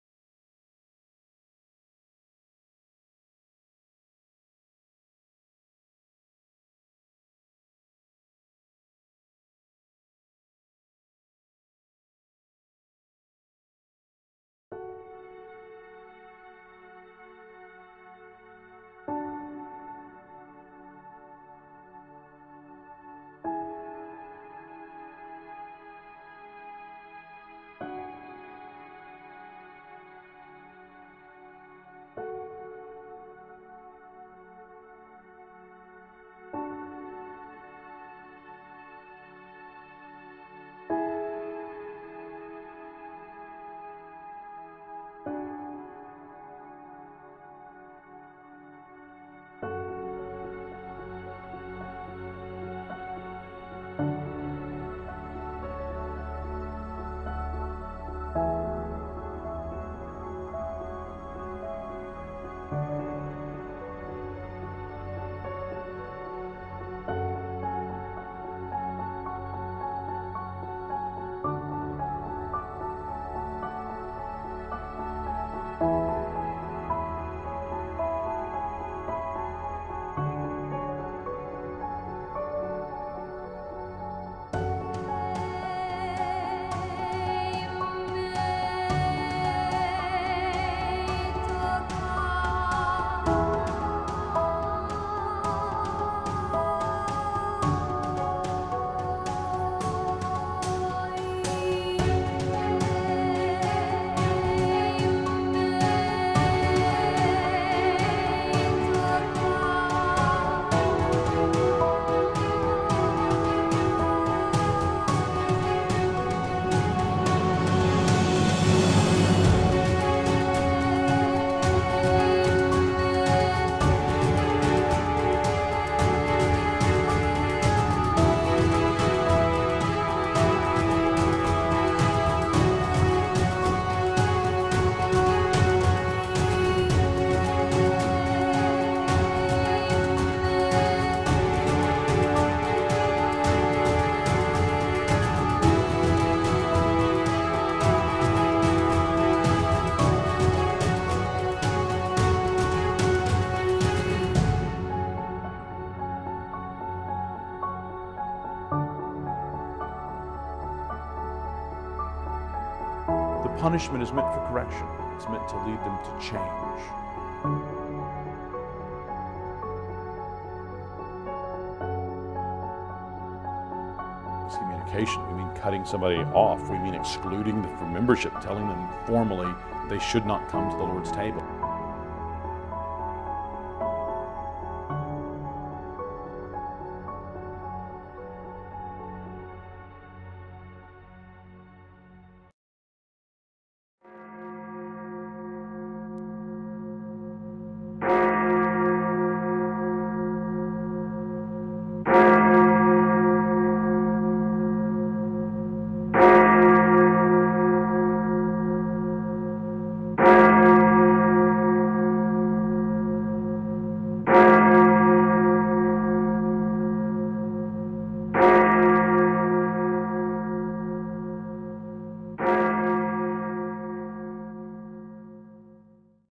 9Marks: Calvinist Takeover. Problems in the Baptist Church
How to convert a church to 9Marks Calvinism by Mark Dever.  9Marks is a dangerous system that seeks to enslave congregations through the use church contracts, discipline, and intimidation.  The 9Marks group masks their Calvinism while deceptively promoting a blending of Calvinist church polity with Baptist congregationalism.  Once a church has fallen for the 9Marks system it is nearly impossible to stop the inevitable Calvinist takeover.  For more information on how to stop the fight, please